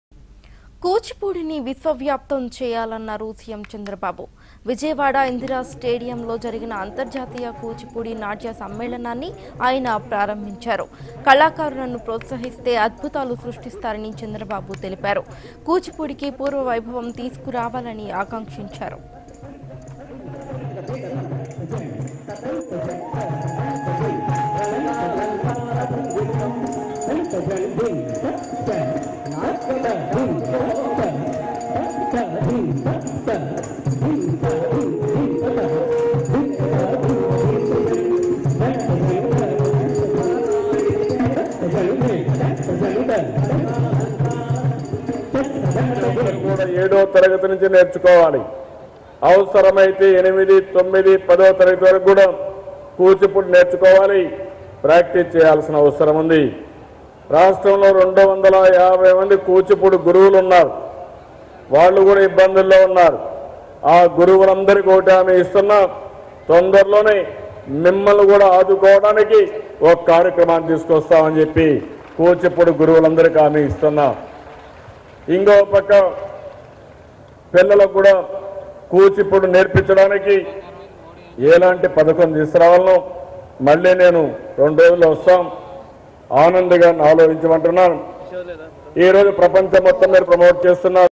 AP CM Chandrababu Naidu Attends International Kuchipudi Dance Convention | Vijayawada
AP CM Chandrababu Naidu Attends International Kuchipudi Dance Convention | Vijayawada | Bhaarat Today